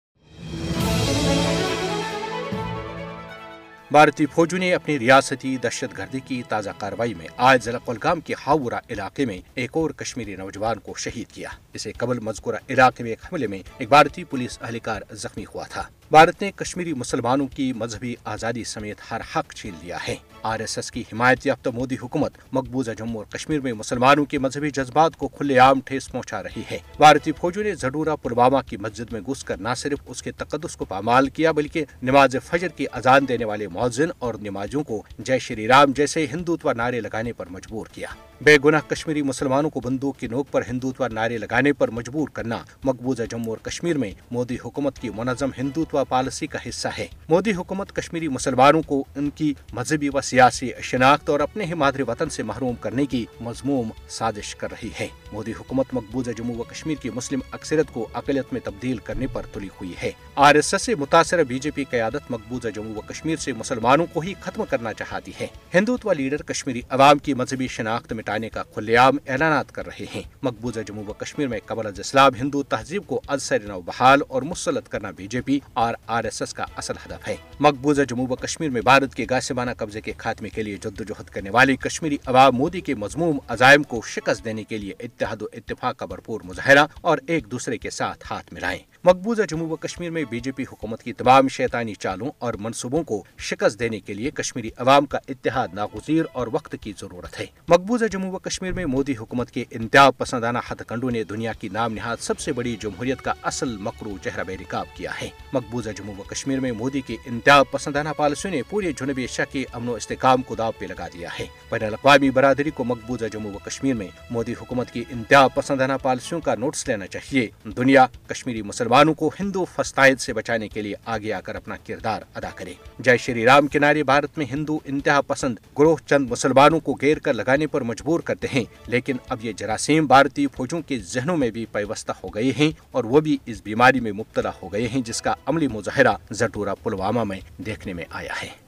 Modi’s Hindutva policies in IIOJK have endangered peace & stability in South Asia | Urdu | RSHJ&K
Modi’s Hindutva policies in IIOJK have endangered peace & stability in South Asia | Urdu | Radio SHJ&K